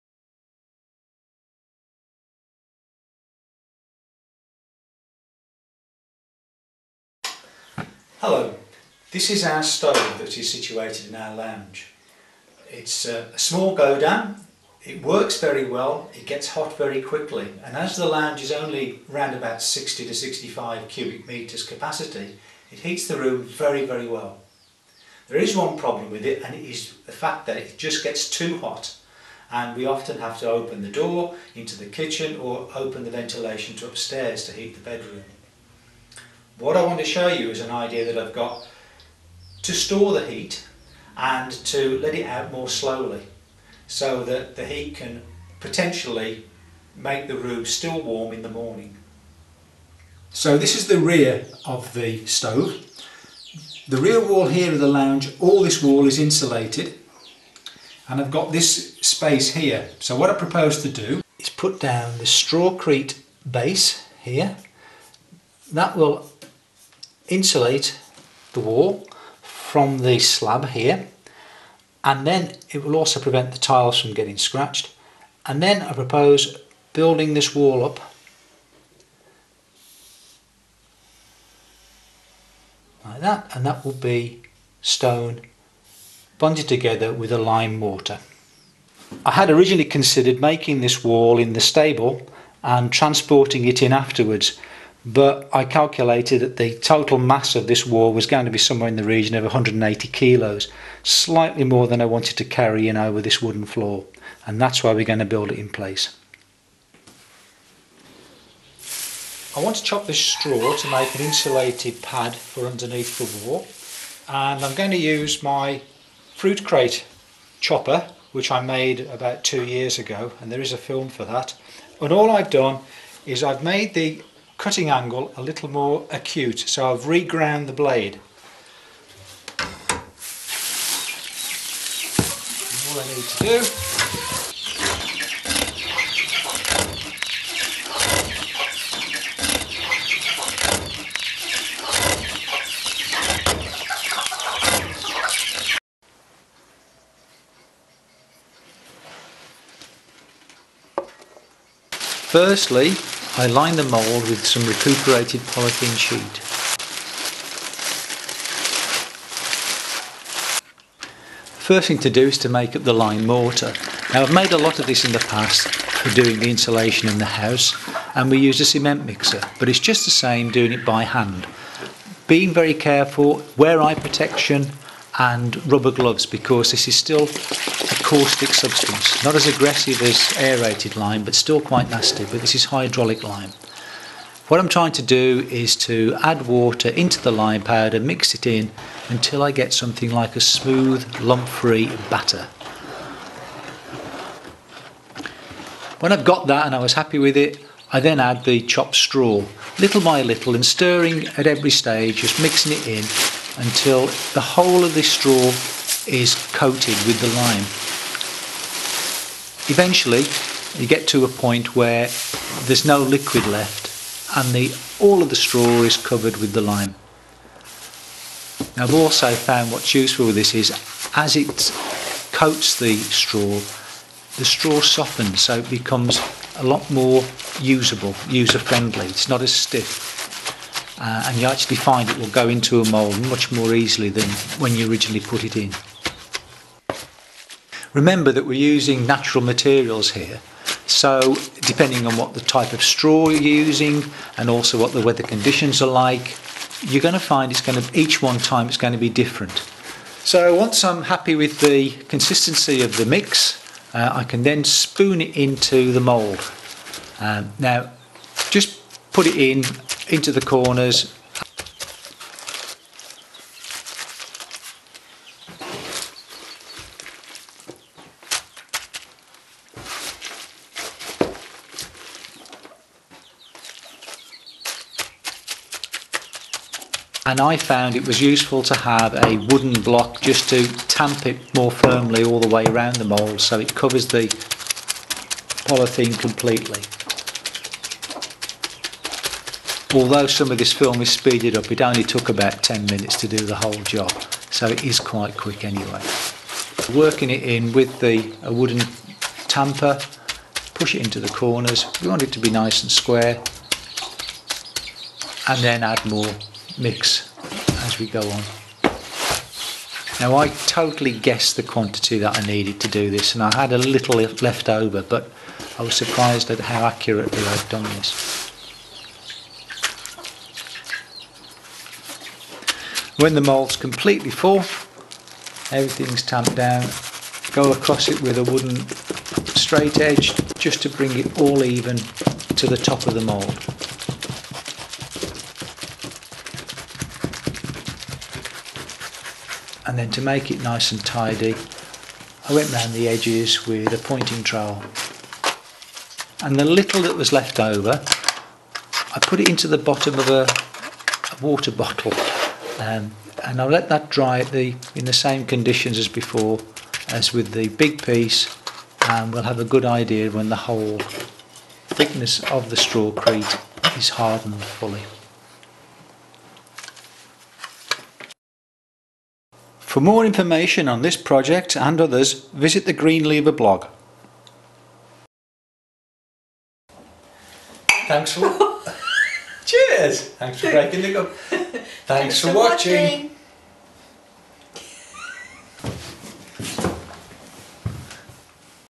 Stone/strawcrete storage heater for our wood stove. Brico ecolo radiateur Bricolaje calentador
To save energy and store heat from our wood stove, we came up with the idea of using local organic triticale straw and stone from our garden to create a stove heat sink or storage heater. There are written and photographic step-by-step articles to accompany this project starts with the strawcrete pad and follows which starts the build.  Our home-made guillotine shown in the film can be found
Para ahorrar energía y almacenar calor, se nos ocurrió la idea de utilizar la paja de triticale orgánica y piedra de nuestro jardín para crear un disipador de calor estufa o calentador de almacenamiento. Nuestro guillotina casera se muestra en la película se puede encontrar y todos los detalles de este proyecto que incluye más información sobre los materiales y las cantidades se puede encontrar
Pour économiser de l'énergie et de stocker la chaleur, nous sommes arrivés avec l'idée d'utiliser la paille de triticale bio locale et de la pierre de notre jardin pour créer un dissipateur de chaleur du poêle ou un radiateur à accumulation. Notre guillotine artisanale montré dans le film peut être trouvée et tous les détails de ce projet, y compris plus d'informations sur les matériaux et les quantités peuvent être trouvés